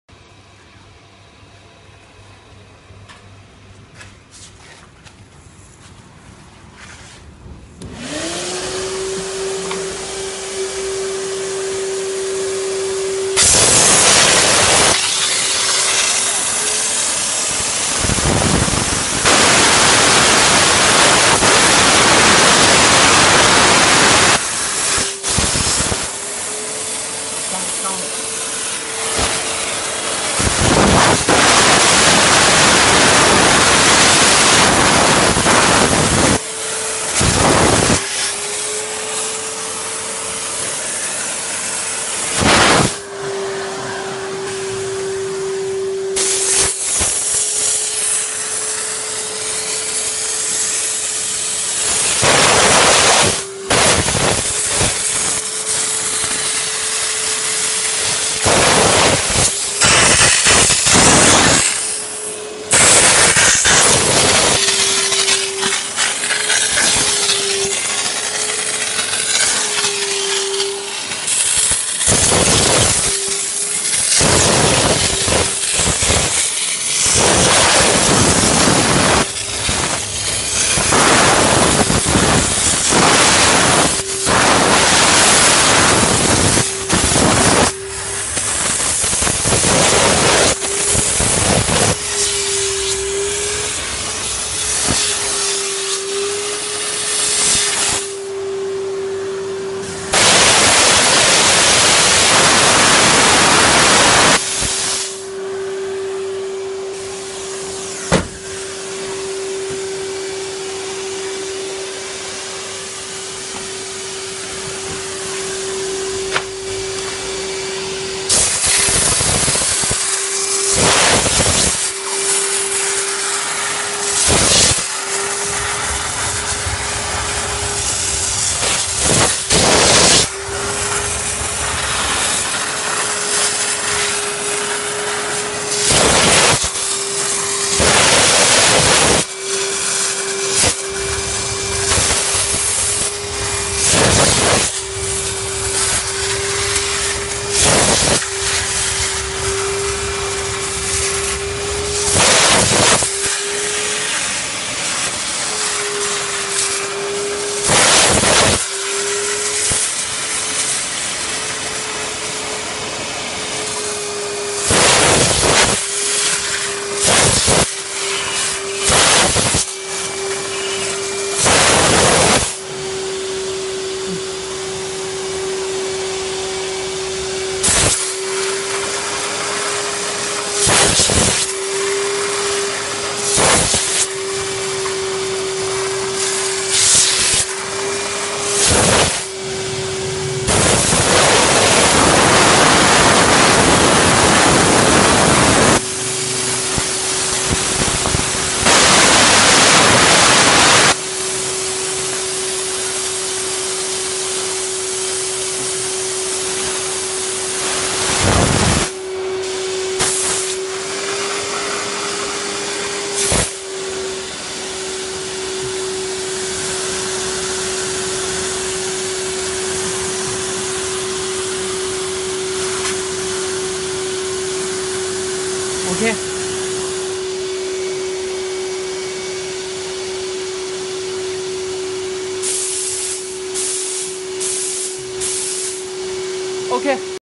diesle model clean car inside with vacuum
Our steam cleaner can connect with a vacuum cleaner for cleaning car inside, carpet and mat.($200/set for vacuum)
We have a steam vacuum nozzle, it can steam cleaning and dedusting at the same time.